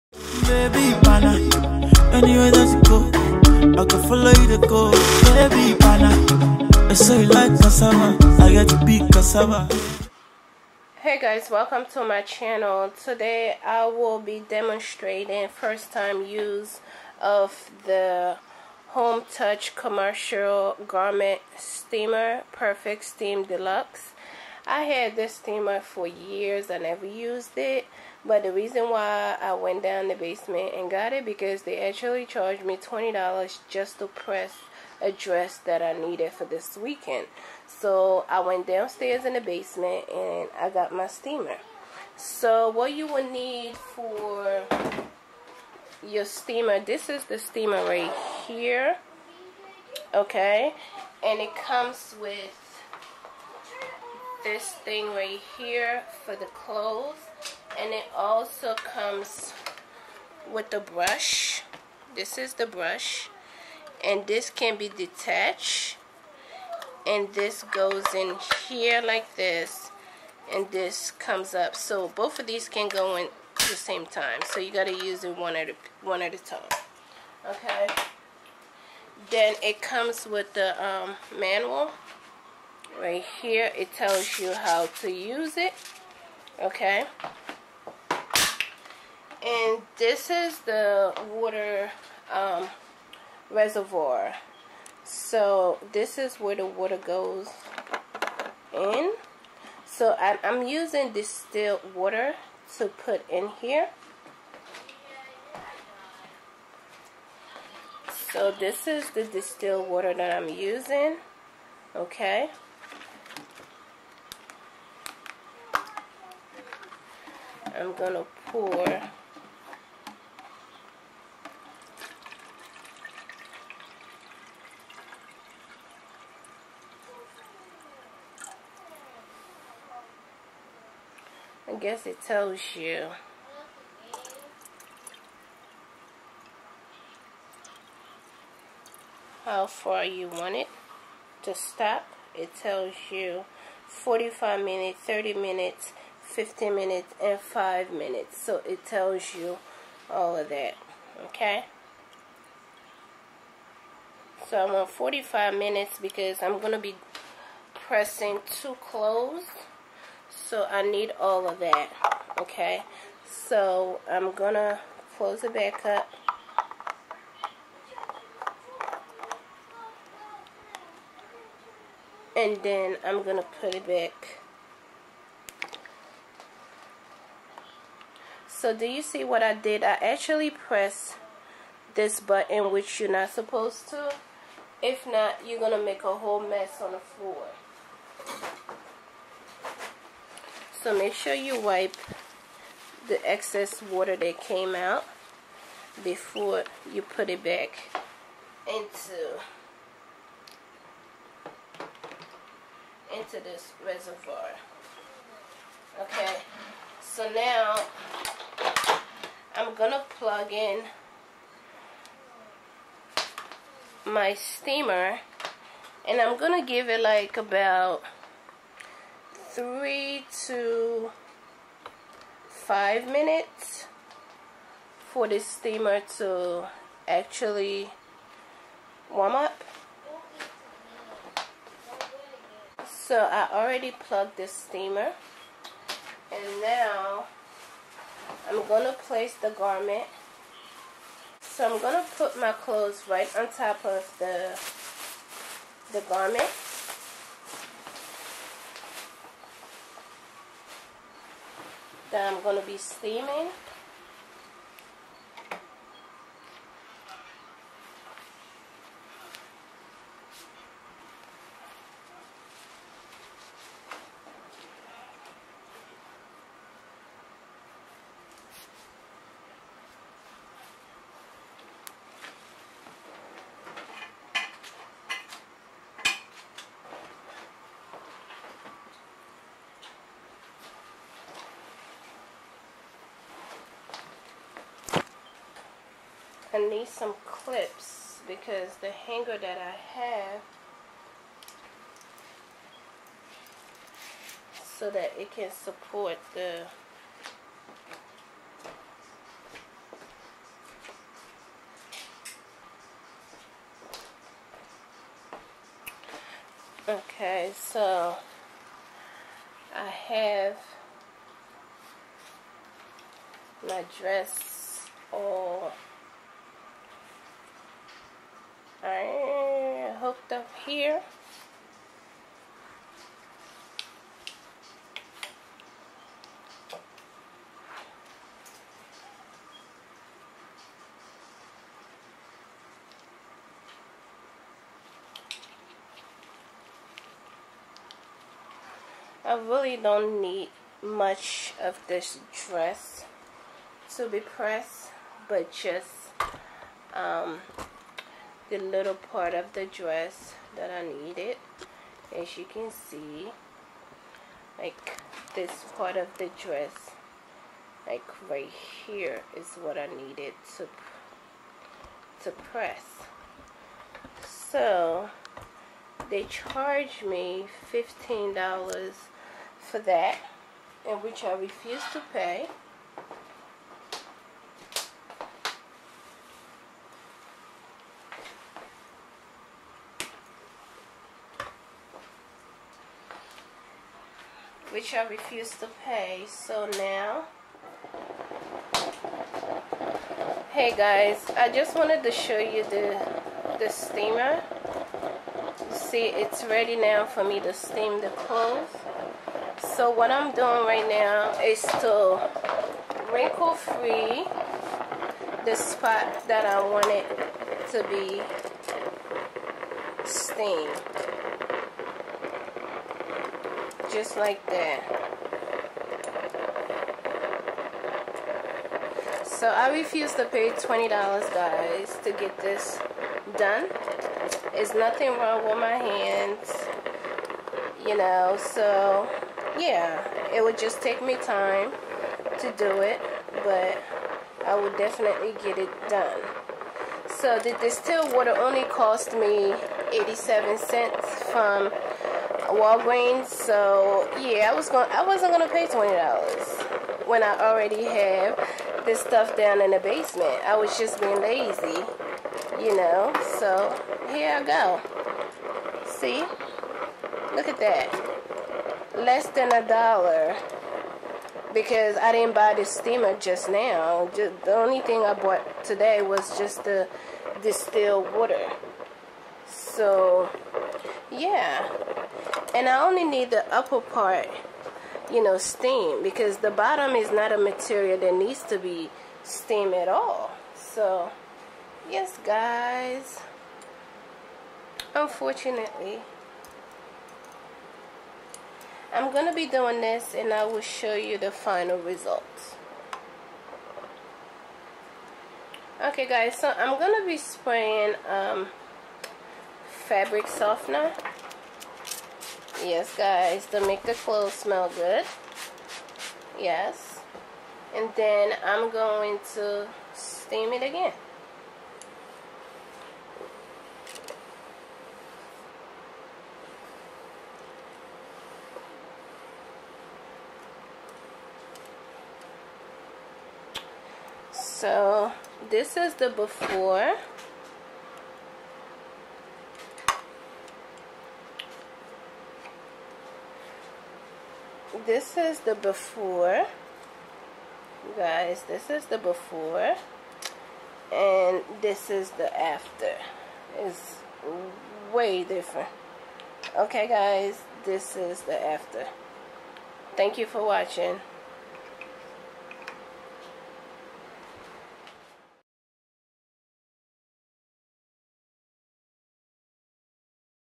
How To Steamed Your Clothes At Home For Less Than A Dollar!
Hey everyone, I got this steamer couple years ago as a gift but never used it. In this video I demonstrated how to use it and it cheaper than the dry cleaners. Let me know what you think about this item.

Product mentioned

1. Distilled water.89 cent
2. Steam machine not too sure how much they costs now, but will check later at Macy’s.com for this product.